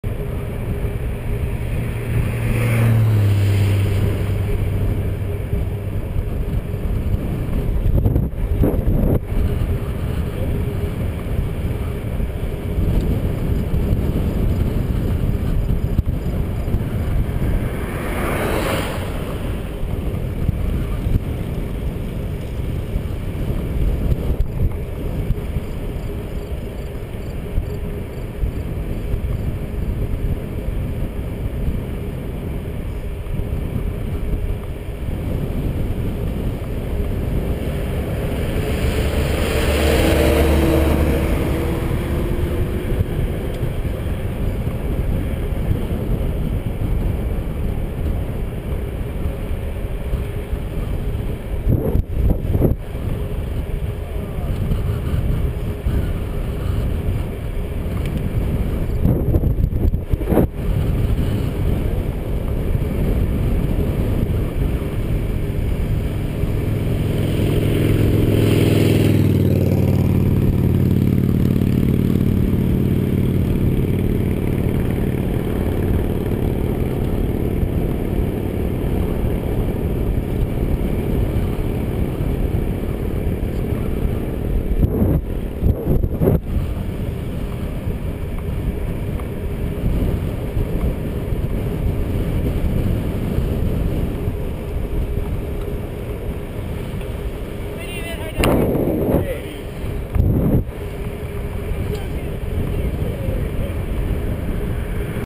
Ride the Rideau 2010 - Coming into Kemptville
Ride the Rideau in support of cancer research at the Ottawa Hospital Research Institute.